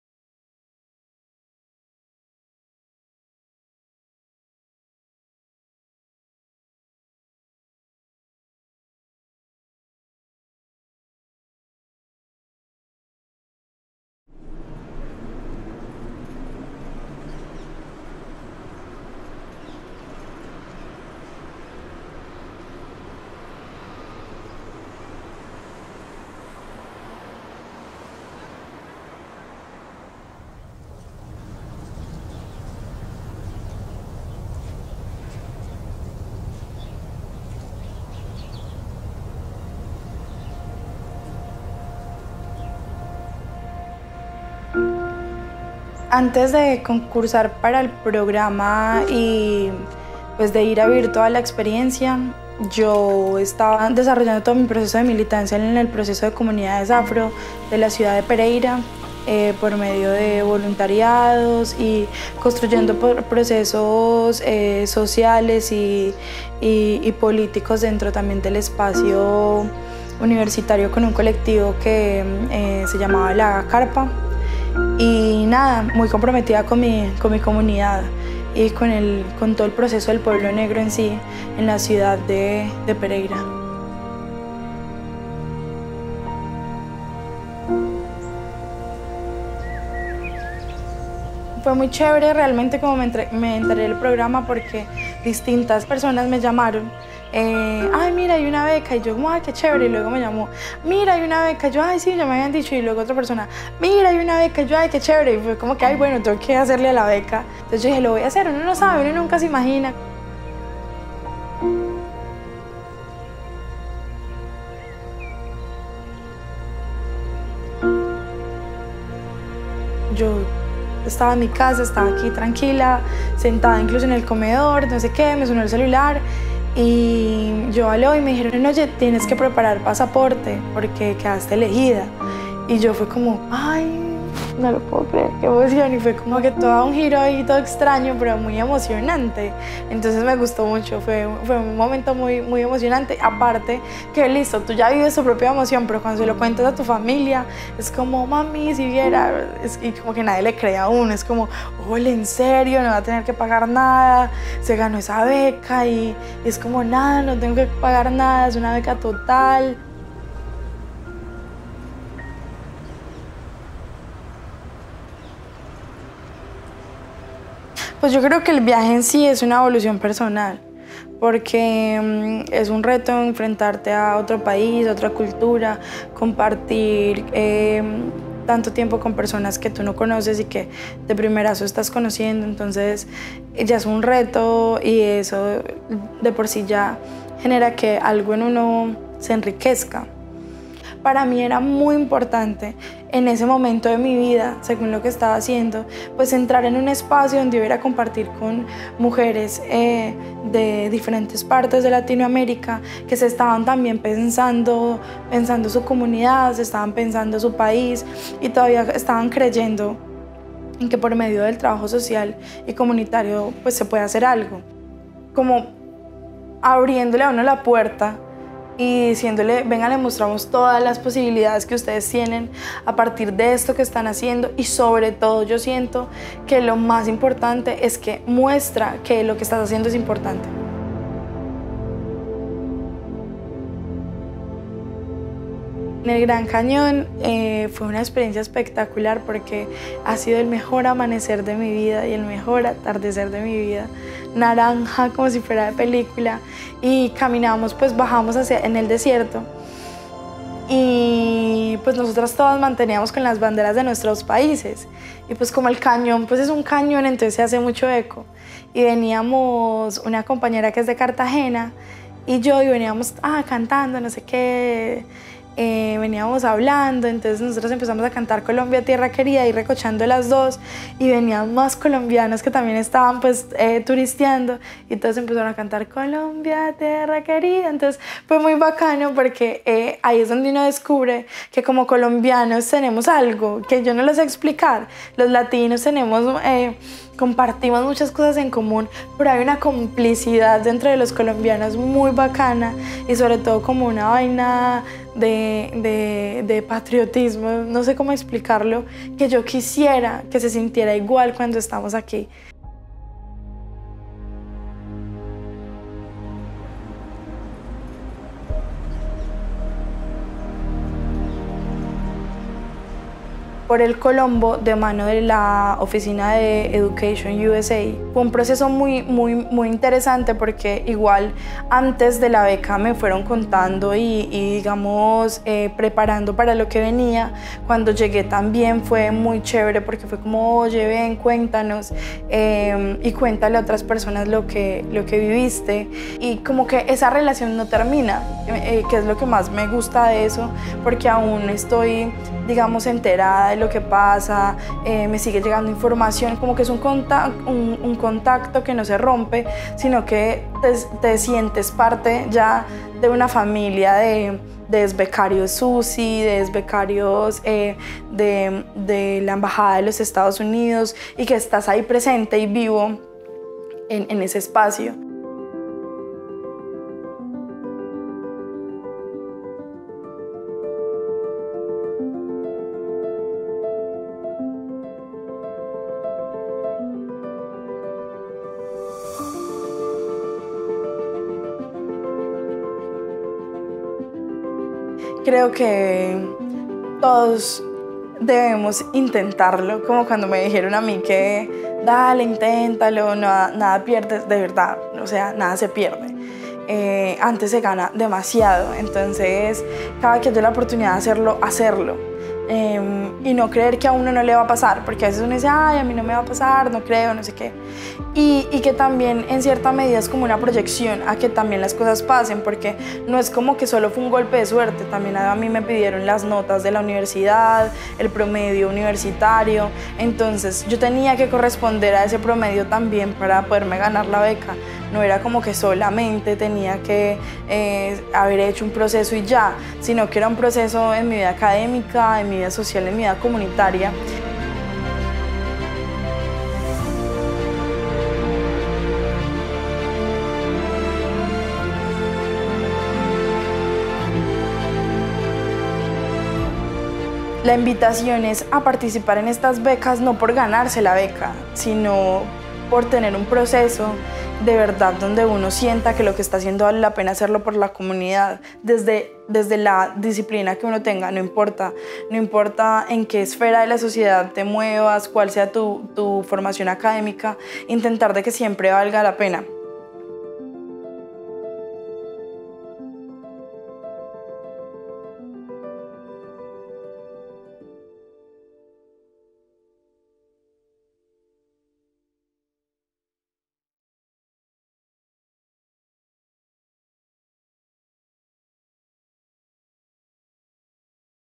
Miniserie EducartionUSA - Nayila
Nayila Liv, quien estudió en la Universidad de Arizona, Tucson, logró complementar y explorar más posibilidades en su vocación comunitaria gracias a su participación en el programa SUSI.